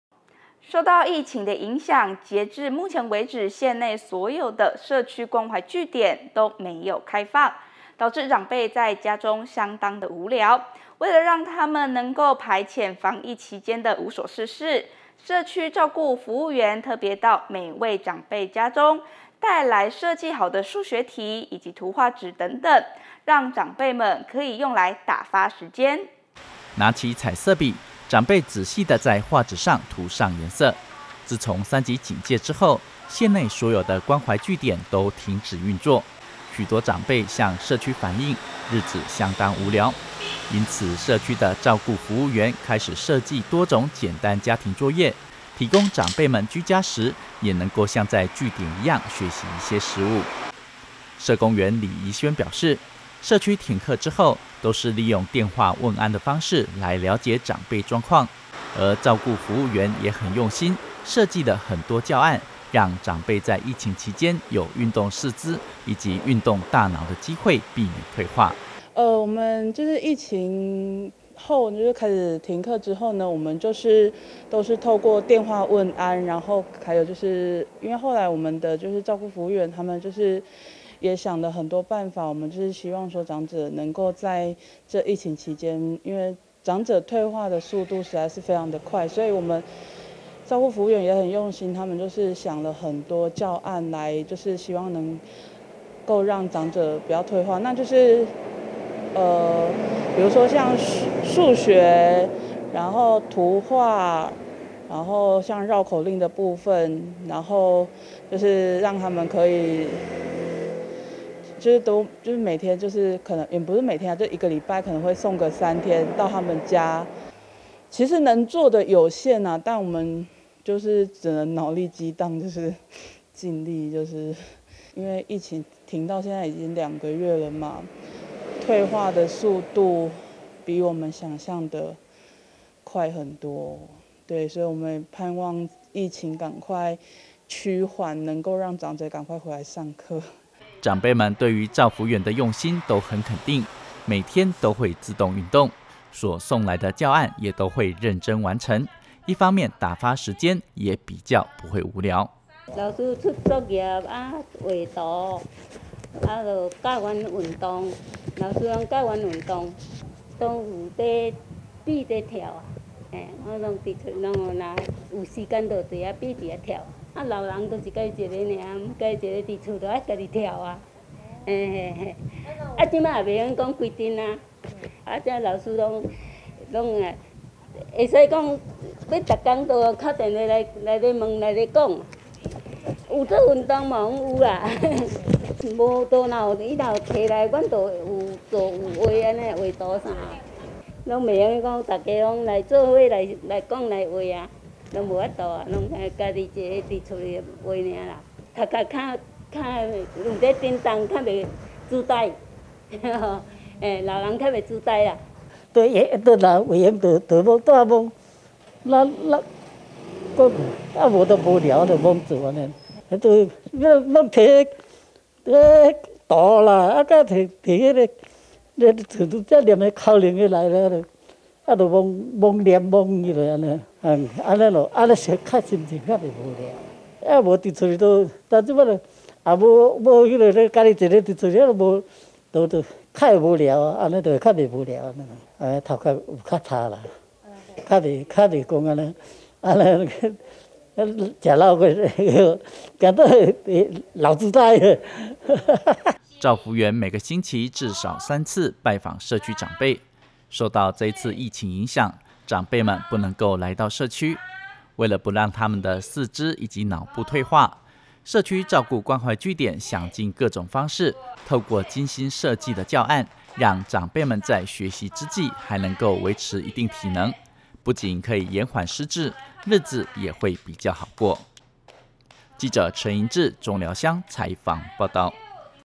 訪視社區長輩 照服員備作業讓長者動腦｜南投縣政新聞 2021.08.04
受到疫情的影響，縣內社區懷據點暫停開放，長輩在家中沒有生活重心，為了要讓他們能夠排遣防疫期間的無助，社區照顧服務員特別到每位長輩家中，帶來設計好的數學題目、圖畫紙等，讓長輩們打發時間並且增加頭腦靈活。